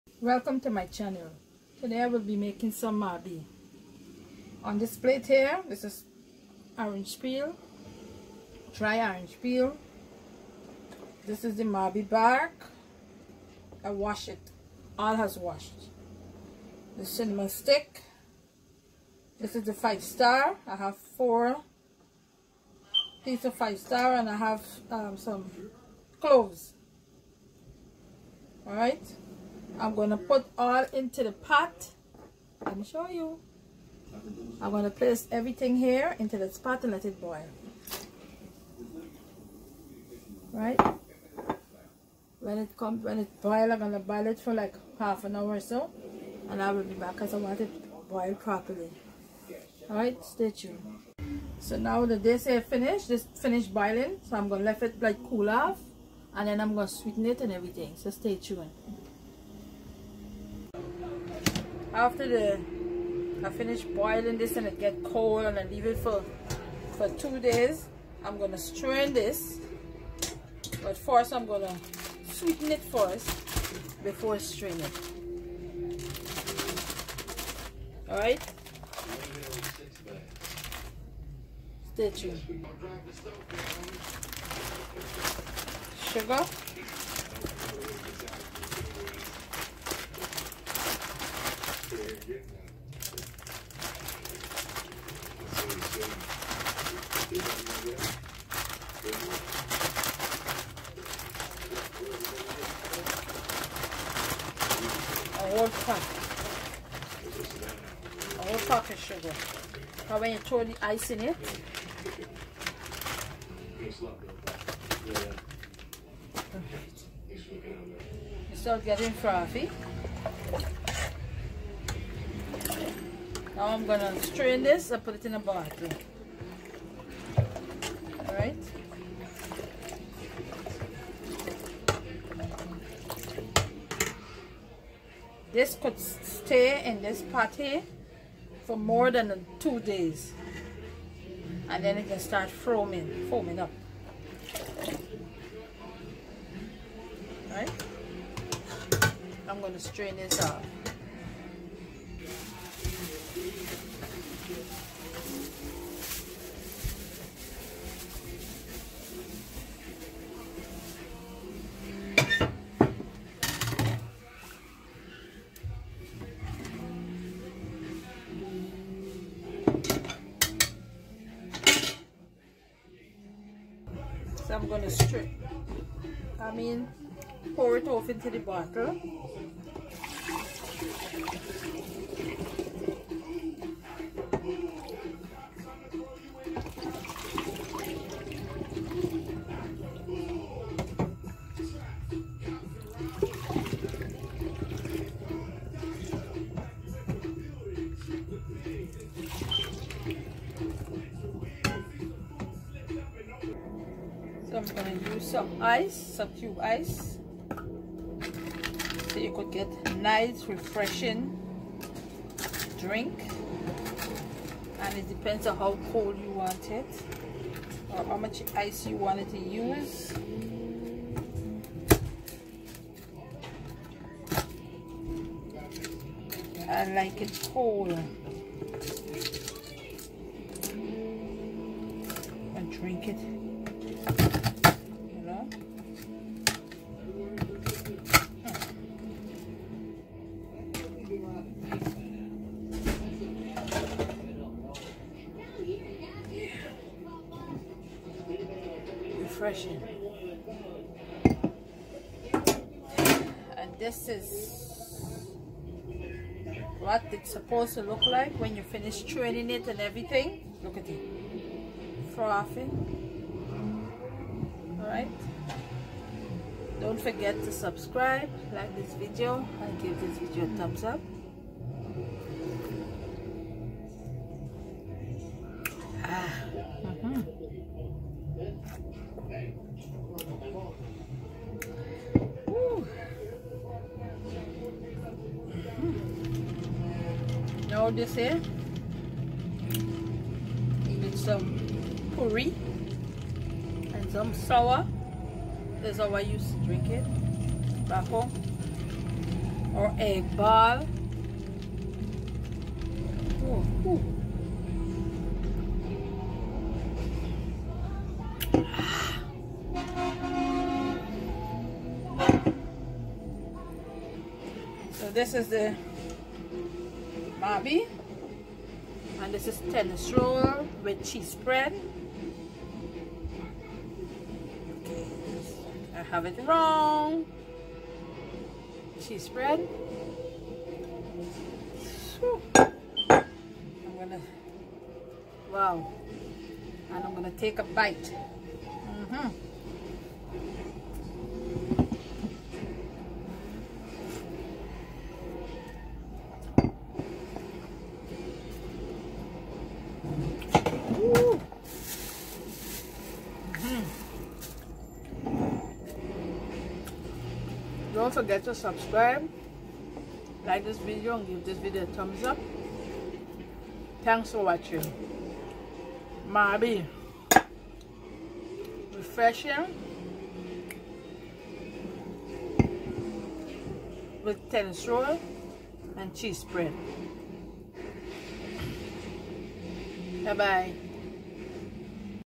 Niice & Refreshing 🇬🇾Homemade Mauby Guyanese 🇬🇾 style
Mauby stick
 Spice & clove
 5 star
1 pack sugar

Your girl yevette love you ❤🇬🇾❤🇬🇾❤🇬🇾❤🇬🇾❤